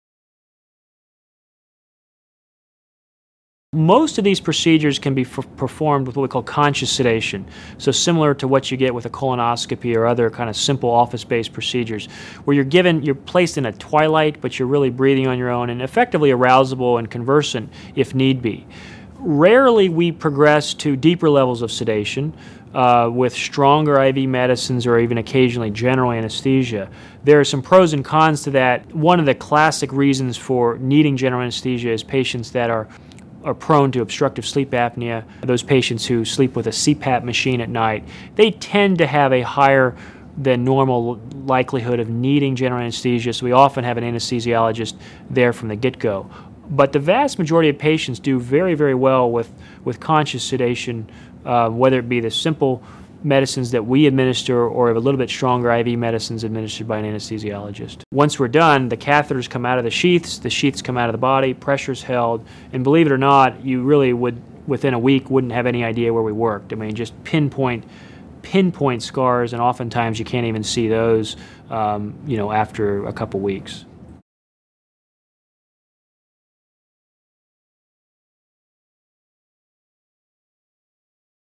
What types of sedations are used during atrial fibrillation catheter ablation?
Dr. Darryl Wells of the Swedish Heart and Vascular Institute discusses the different types of sedation used during catheter ablation. For more about atrial fibrillation and catheter ablation procedures